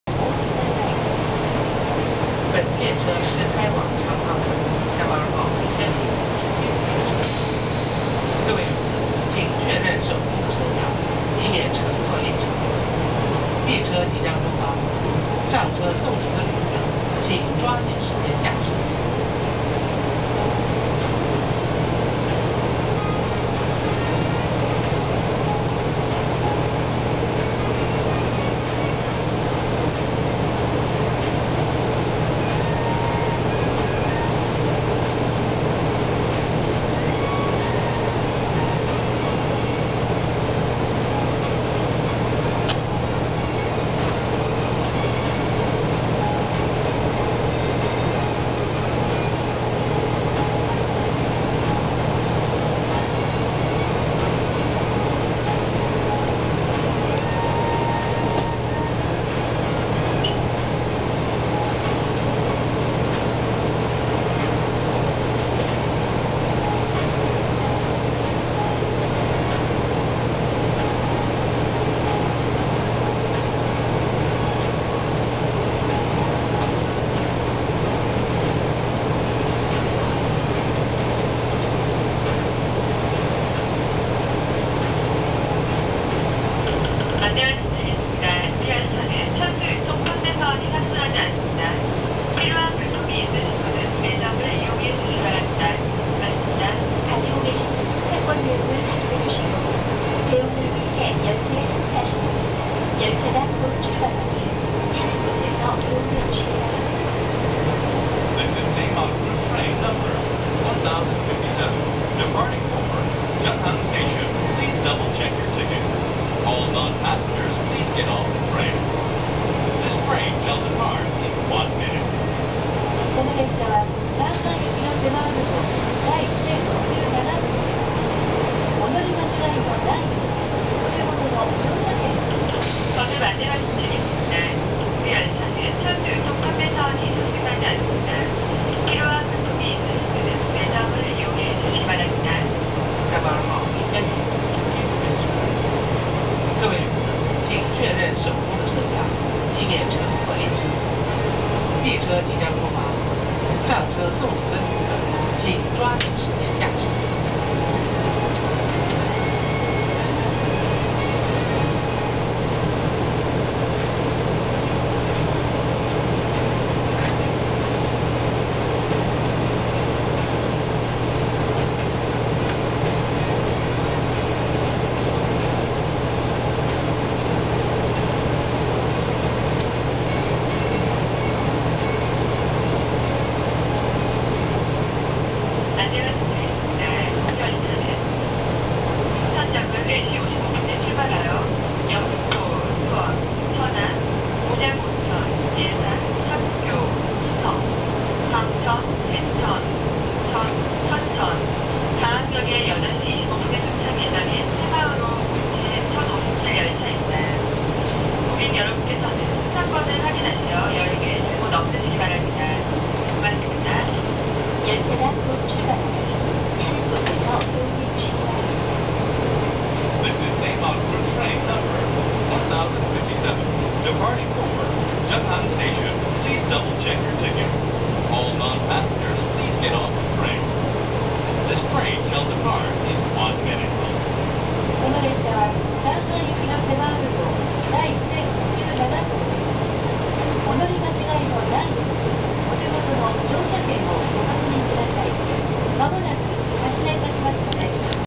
Saemaul train's video animation
This is the video animation you could see in the Saemaul train before it started running at the initial station, or it had just arrived the terminal station. One could see this until the late 2007.

South Korea's Saemaul train is known as the first in the world to introduce video screens in passenger cars in 2000.

Nowadays KTX does offer pre-running video animation as Saemaul train have done, but it's significantly slenderer than the Saemaul's.